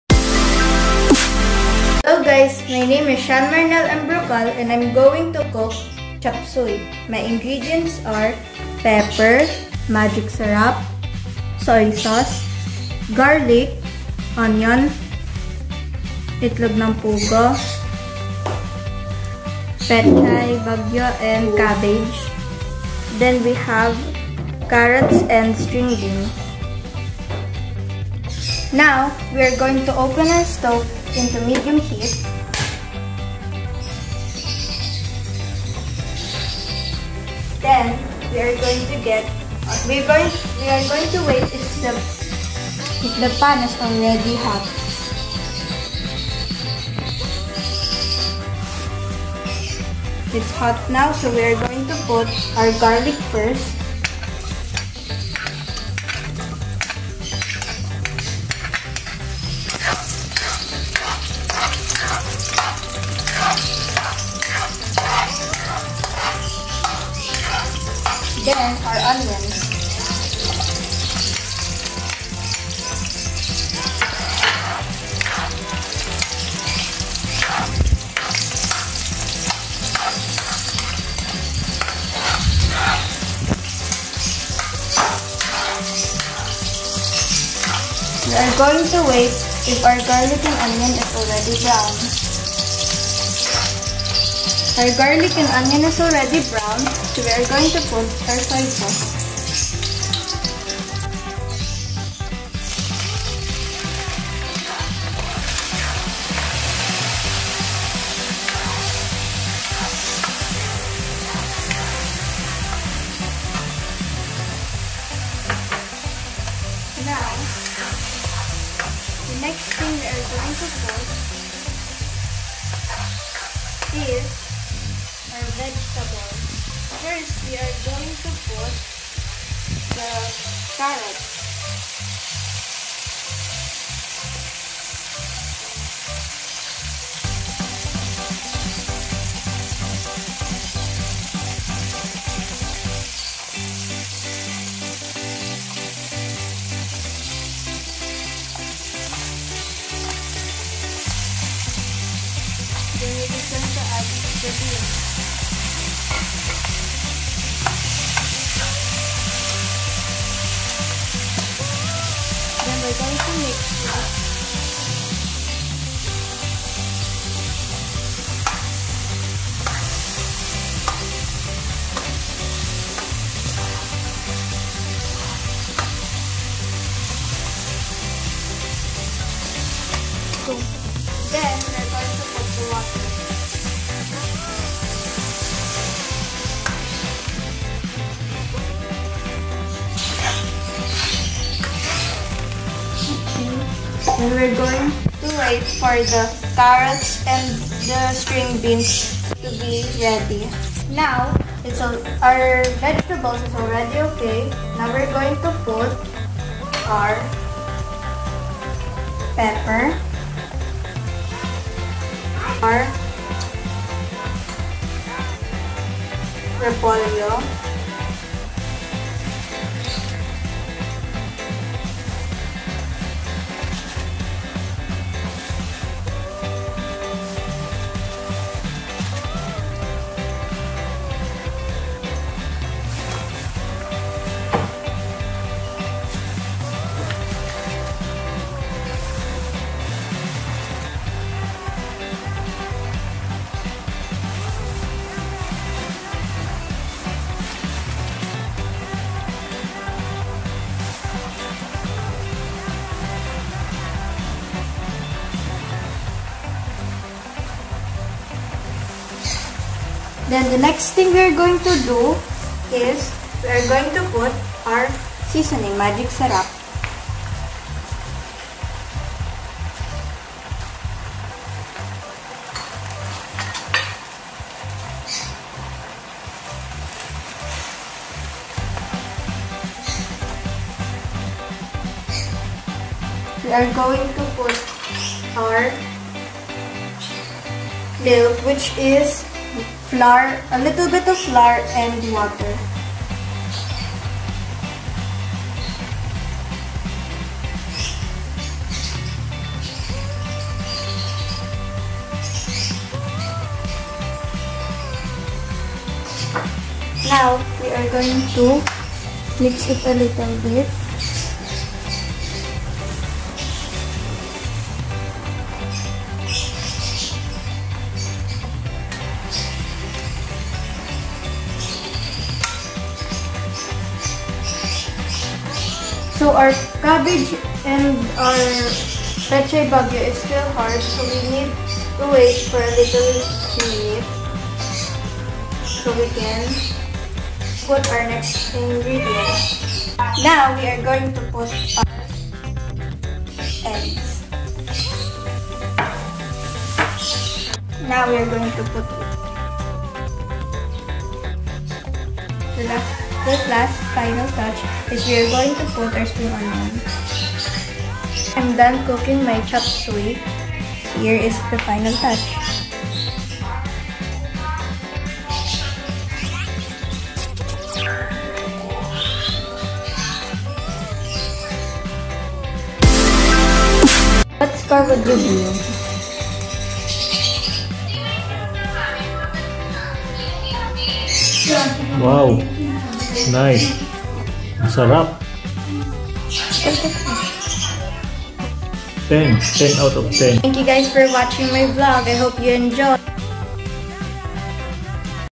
HOW TO COOK CHOPSUEY - PERFORMANCE TASK
Cooking abilities may aid persons in adhering to nutritional recommendations in their regular diet. They enable people to make more nutritious eating choices. As a result, it is critical to teach children and teenagers how to cook and to encourage them to improve their culinary talents.

Performance Task

Grade 6- Rizal

Good Job!